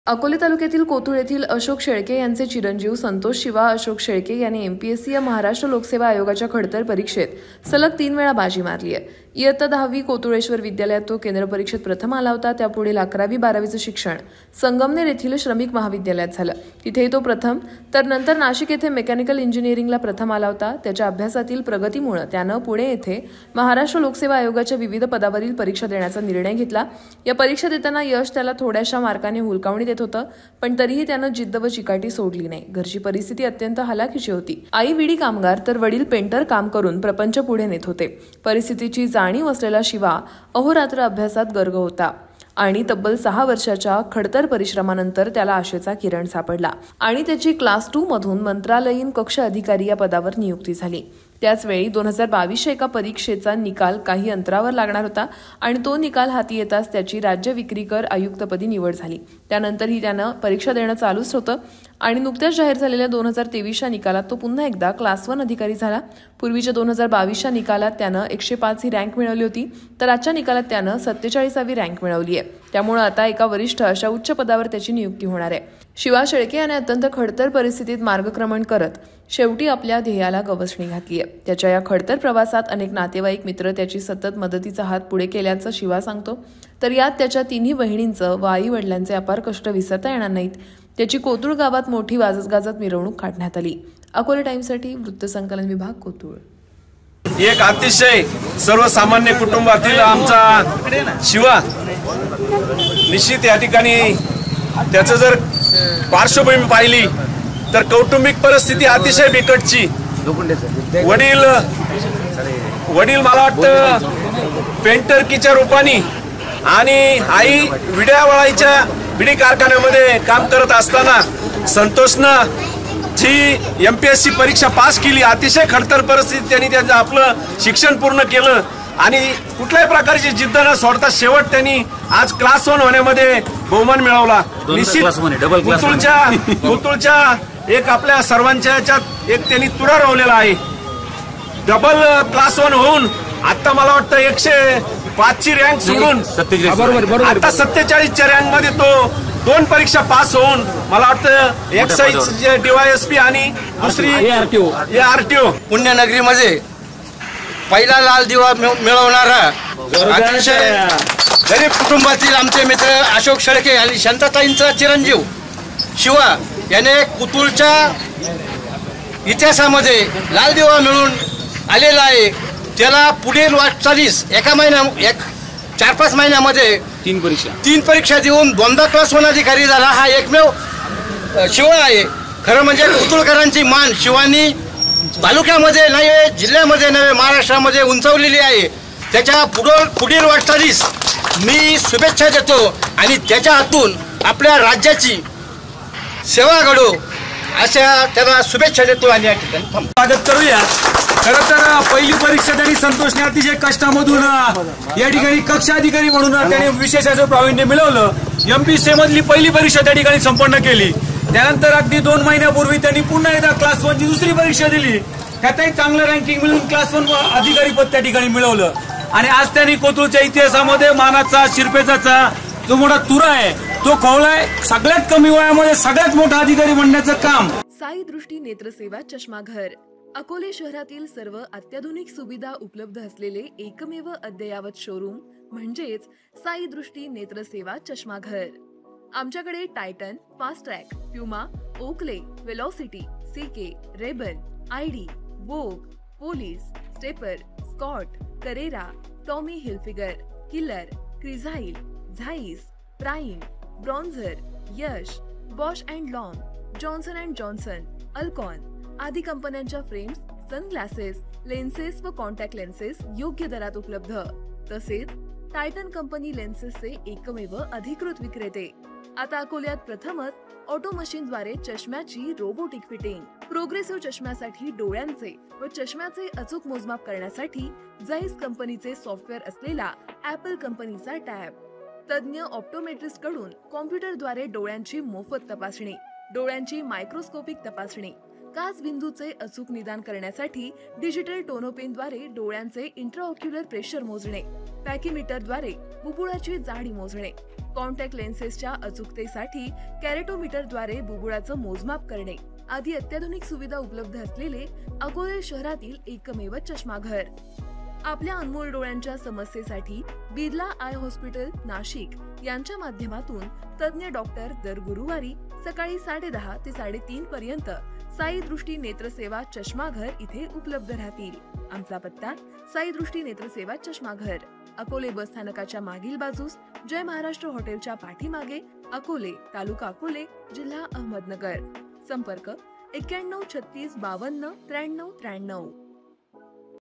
कोतूळचा शिवा शेळके सलग तीन वेळा महाराष्ट्र लोकसेवा आयोग परिक्षा उत्तीर्ण | Akole Times
बिड्या बांधणाऱ्या आईचा मुलगा व पेंटर काम करणाऱ्या बापाचा पोरगा झाला "साहेब"

नव-नवीन अपडेट्ससाठी आमचे फेसबुक इंस्टाग्राम पेज फॉलो करायला विसरू नका

- मा.श्री.अमोल शिर्के
संपादक,अकोले टाईम्स